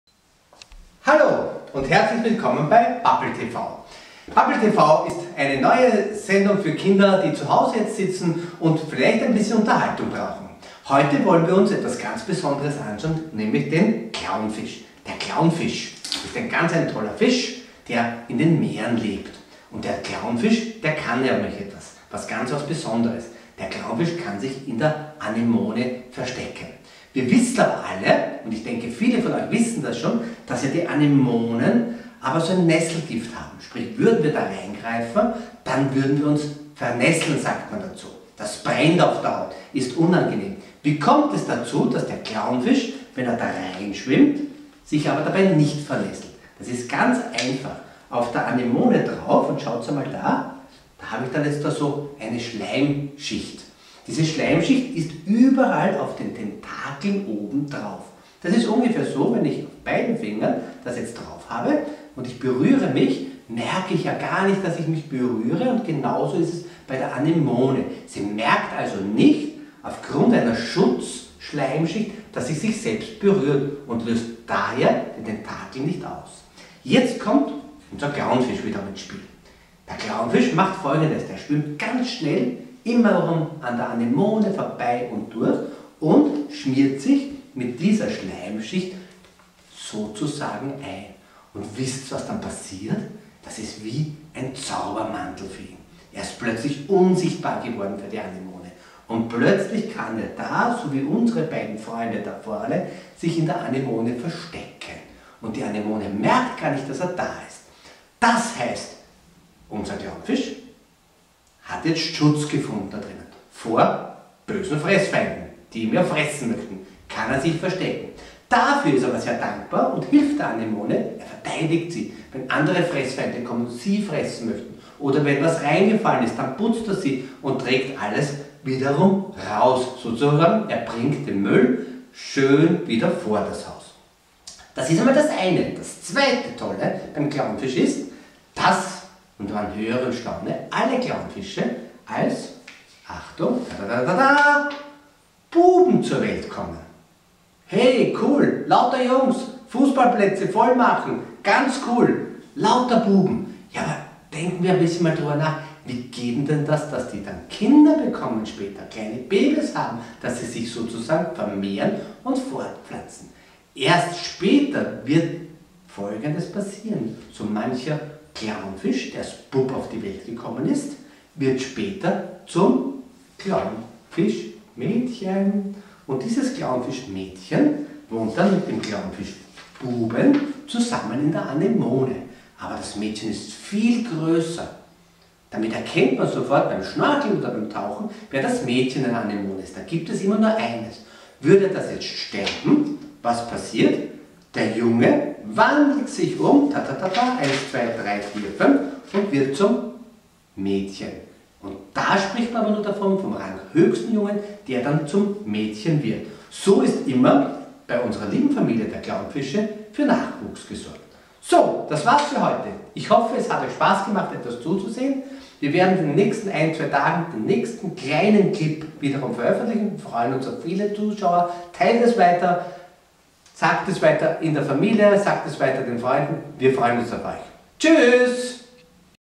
Bubble TV ... Der Clownfisch
Herzlich Willkommen bei Bubble TV...
Bubbel TV ist ein GRATIS Unterhaltungskanal der Dive Company um den Kindern, in der Zeit von Corona, etwas Spaß nach Hause zu liefern!
Heutiges Thema, der Clownfish!